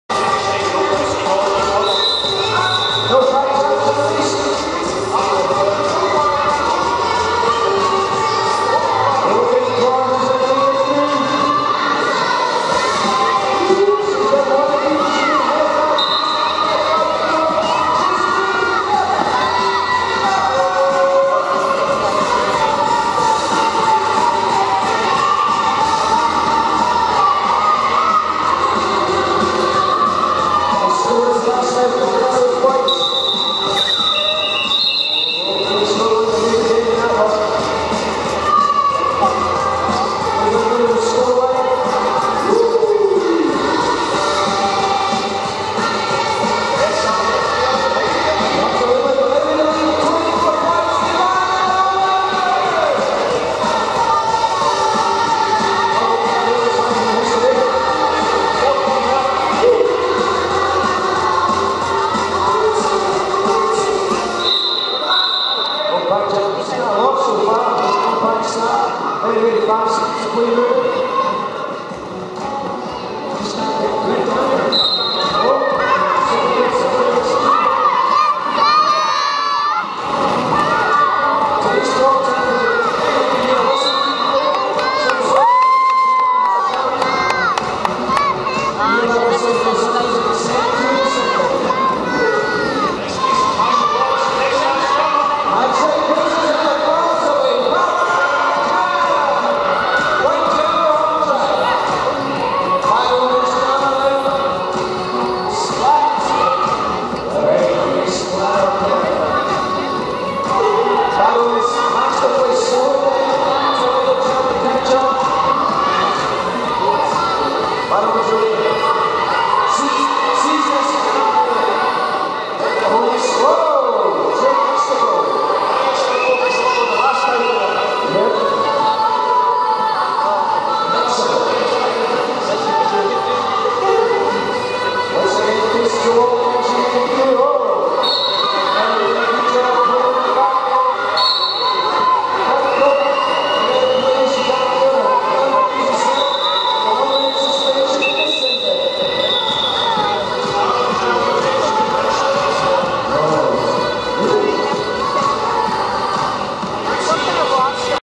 Glasgow Roller Derby 2012 Season: Bad Omens v Death Stars Jam5,6 Period1
Glasgow Roller Derby Home Season 2012
Bad Omens v Death Stars
30 June 2012 @ ARC, Glasgow Caledonian University

This is the 5th and 6th Jams of the 1st Period.

Your jammers are:
Hardcore Prawn (88mph) and (The Very Hungry) Splatterkiller (0) for the Bad Omens
Rogue Runner (210) and Marshall Lawless (42) for the Death Stars
Jam 6 is a Omens power jam.